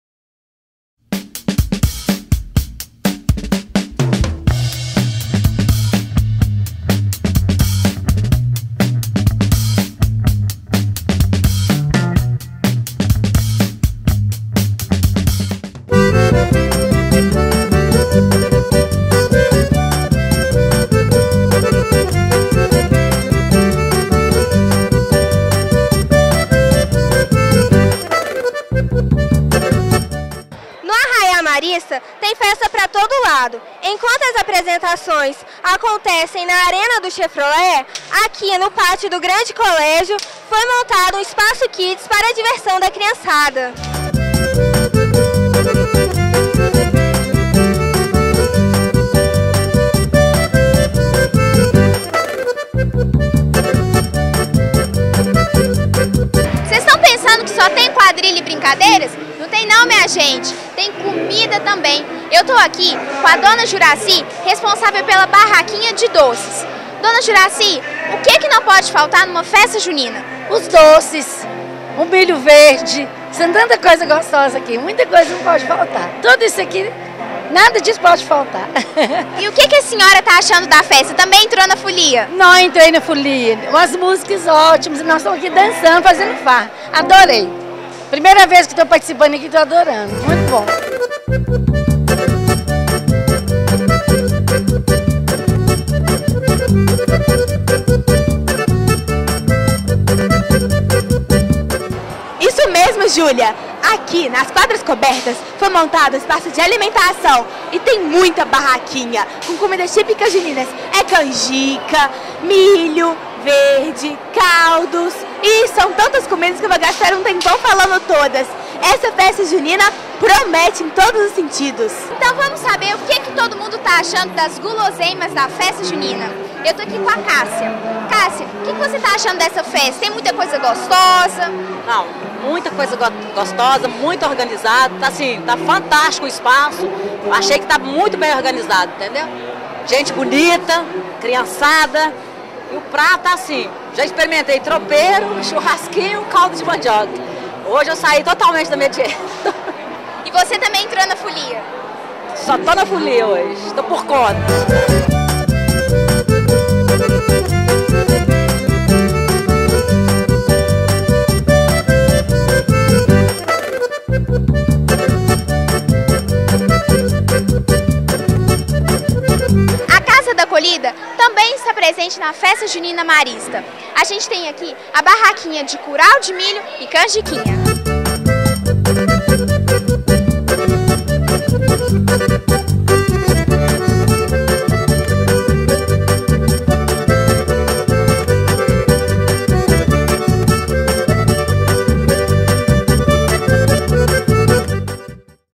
Festa Junina 2013 | Parte 4
| Depois de quase três meses de preparação, o espetáculo da Festa Junina encantou os pais que acompanharam tudo na arena do Chevrolet Hall. Organizada pelo SeAC, Serviço de Arte e Cultura do Colégio Marista Dom Silvério, a festa teve como tema "O Poetinha na Quermesse de Pai Francisco e Mãe Catirina", em homenagem aos 100 anos que o poeta Vinícius de Moraes estaria completando e o centenário da Festa de Parintins, no Amazonas.